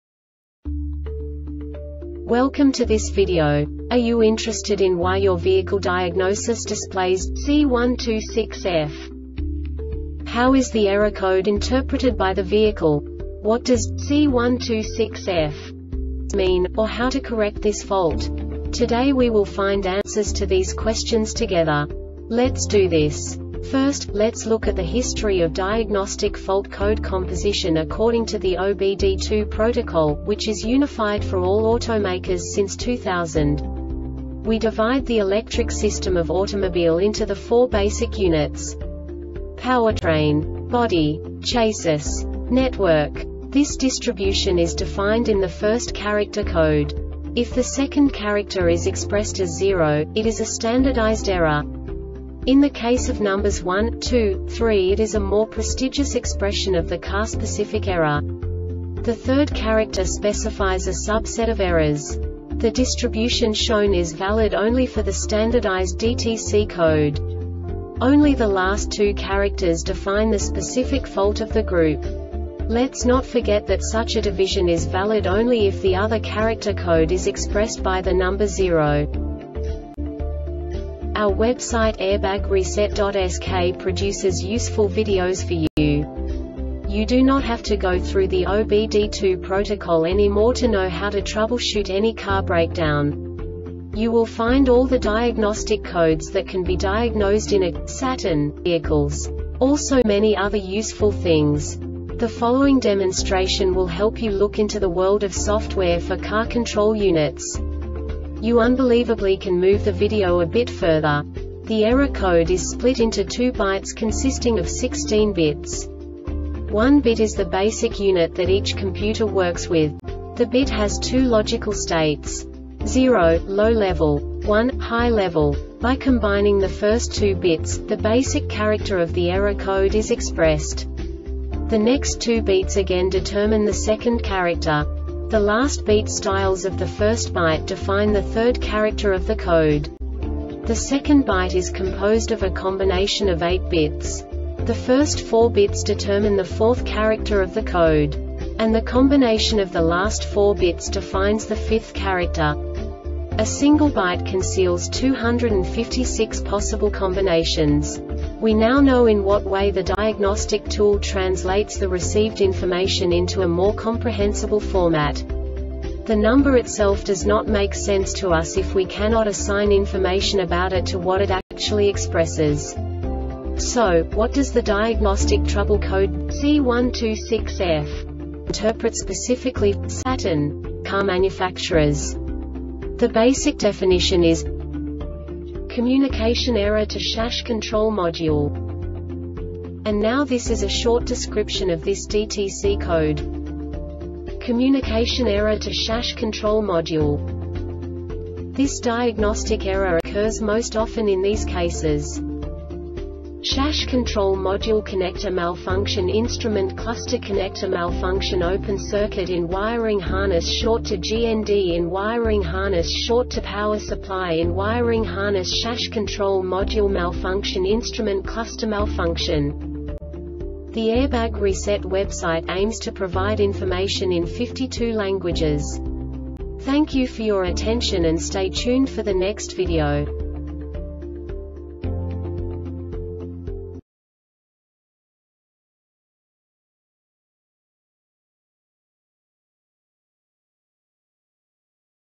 DTC Saturn C126F Short Explanation
Contents:
0:21 Basic DTC analysis according to OBD2 protocol standard.
1:48 Insight into programming
2:58 A diagnostically understandable description of the Diagnostic Trouble Code
3:05 Basic error definition

Make:
Saturn
Code:
C126F
Definition:
Communication Error to SAS Control Module
Description:
Cause:
 SAS control module connector malfunction Instrument cluster connector malfunction Open circuit in wiring harness Short to GND in wiring harness Short to power supply in wiring harness SAS control module malfunction Instrument cluster malfunction